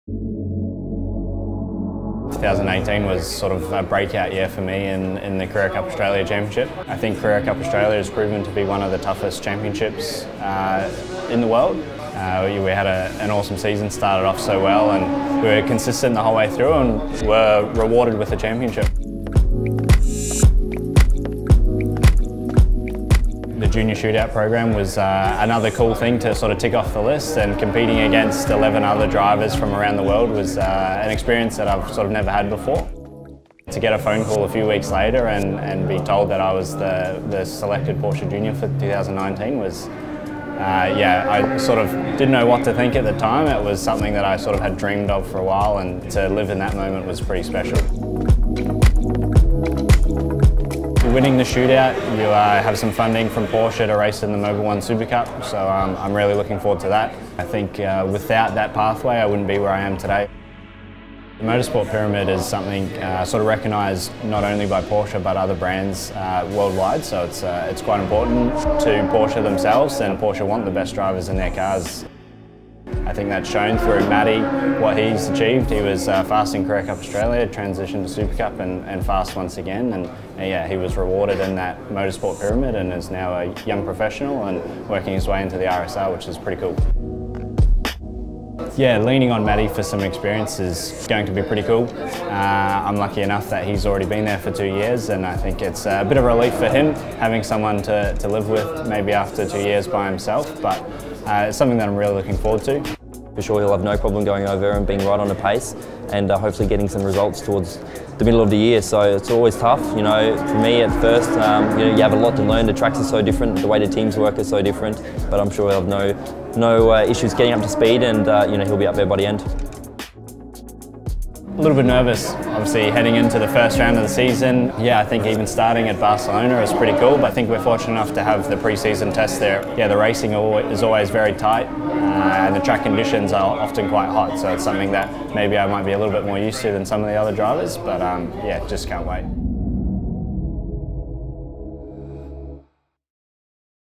Porsche Junior Jaxon Evans begins next level of Porsche Driver Development Pyramid in 2019 Supercup
Porsche Junior Jaxon Evans begins the next step up the Porsche Driver Development Pyramid this weekend when he races in Round 1 of the Porsche Mobil 1 Supercup driving a Porsche 911 GT3 Cup. He follows in the footsteps of fellow Porsche Carrera Cup Australia champion Matt Campbell, who is now a Porsche Young Professional, racing both the Porsche 911 GT3 R and Porsche 911 RSR globally.